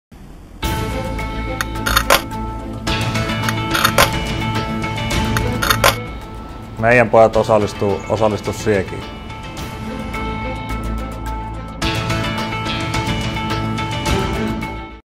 SaiPa - Unicefin Lappeenrannan paikallisryhmän lähettiläs
Unicef saapuu Kisapuiston jäähalliin 9.3.2013 otteluun SaiPa-HIFK keräyslippaiden kanssa. SaiPa on osallistunut, osallistu sinäkin.

Videolla spiikkerinä Ville Koho ja muuten menossa mukana Anssi Löfman, Simon Backman ja Simo-Pekka Riikola.